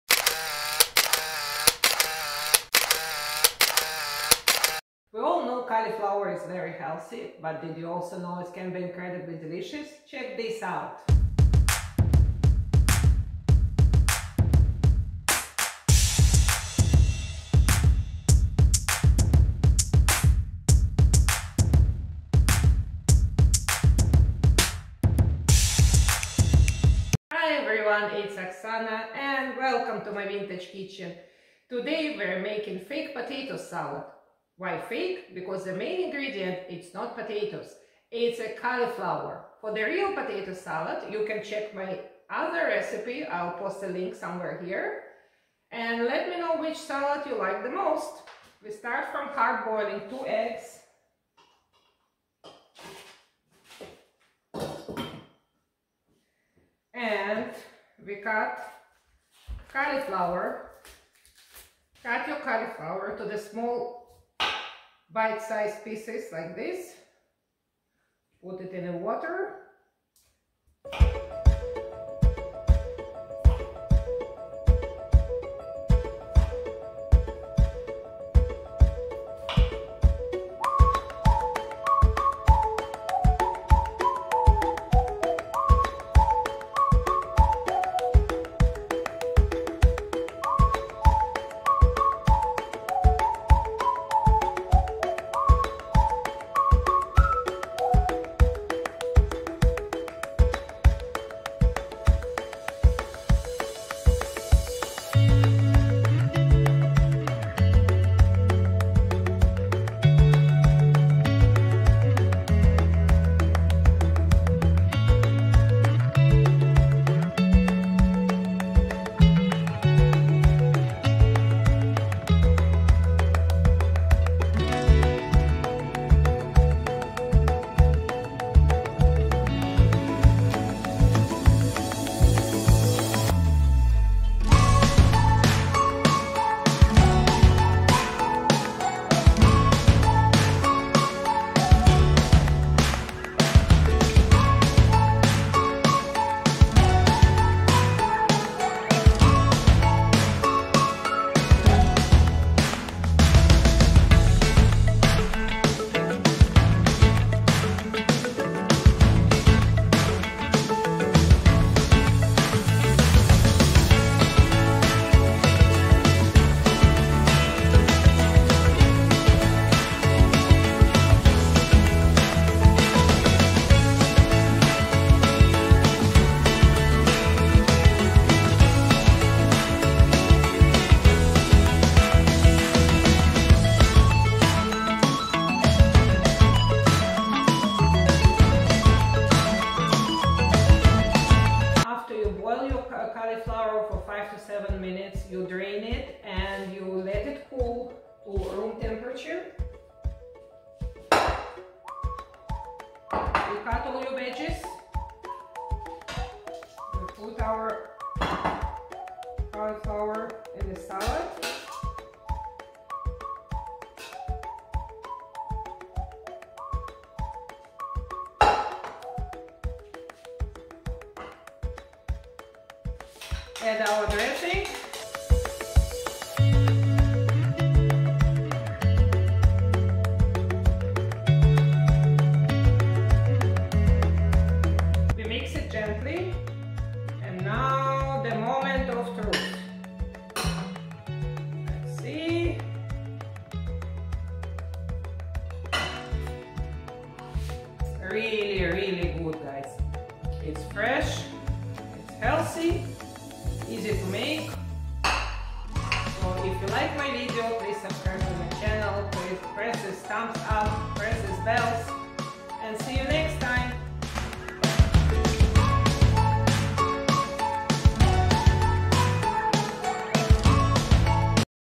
HEALTHY CAULIFLOWER SALAD/ЛУЧШИЙ САЛАТ ИЗ ЦВЕТНОЙ КАПУСТЫ/EASY DINNER IDEAS/
Love potato salad but looking for low carb &  easy dinner ideas? Stop looking, you found it right here. Other than the cauliflower, this low carb potato salad is just like the one you probably used to. It has the classic ingredients but much less calories, keto, gluten-free and healthy cauliflower salad.

Healthy Cauliflower Salad

1 small cauliflower
2 cucumbers
1 pickle
1-2 celery sticks
2 hard boiled eggs
2 tablespoons mayonnaise
1 tea spoon mustard
1 tablespoon lemon juice
parsley

Лучший салат из Цветной капусты. Этот псевдокартофельбый салат станет любимцем на вашем столе. Полезный,низко калорийный,кето, без глютена ! Самое главное очень вкусный и быстро готовится. Что еще нужно летом?

ЛУЧШИЙ САЛАТ ИЗ ЦВЕТНОЙ КАПУСТЫ

1 небольшая цветная капуста
2 свежих огурца
1 соленый огурец
1-2 стебля сельдерея
2 яйца
2 ст.ложки майонеза
1 ч.ложка горчицы
1 ст.ложка лимонного сока
петрушка